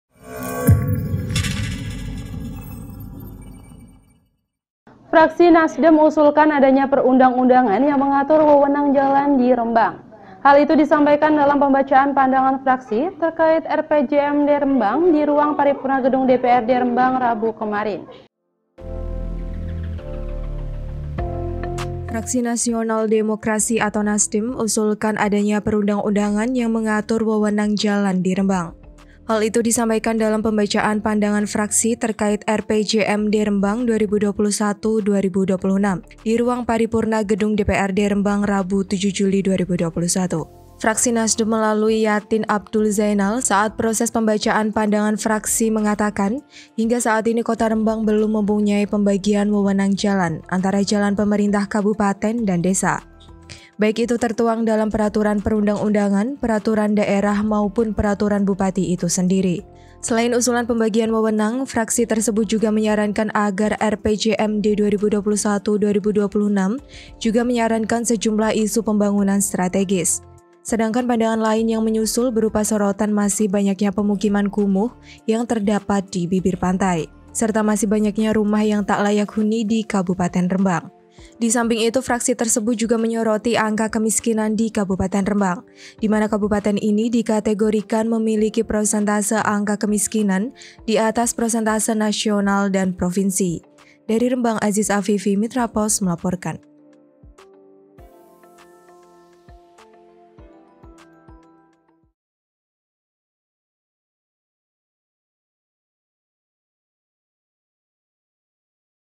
Fraksi Nasdem Usulkan Peraturan Pembagian Wewenang Jalan Kabupaten Dan Desa
Fraksi Nasional Demokrat atau Nasdem, usulkan adanya perundang-undangan yang mengatur wewenang jalan di Rembang.

Hal itu disampaikan dalam Pembacaan Pandangan Fraksi Terkait RPJMD Rembang (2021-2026), Di Ruang Paripurna Gedung DPRD Rembang Rabu (7/7/2021).

Fraksi Nasdem melalui Yatin Abdul Zaenal, saat proses pembacaan pandangan fraksi mengatakan, hingga saat ini kota Rembang belum mempunyai pembagian wewenang jalan, antara jalan pemerintah kabupaten dan desa. Baik itu tertuang dalam peraturan perundang-undangan, Peraturan daerah, maupun peraturan bupati itu sendiri.

Selain usulan pembagian wewenang, fraksi tersebut juga menyarankan agar RPJMD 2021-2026 juga menyarankan sejumlah isu pembangunan strategis.

Sedangkan pandangan lain yang menyusul, berupa sorotan masih banyaknya pemukiman kumuh yang terdapat di bibir pantai. Serta masih banyaknya rumah yang tak layak huni di kabupaten Rembang.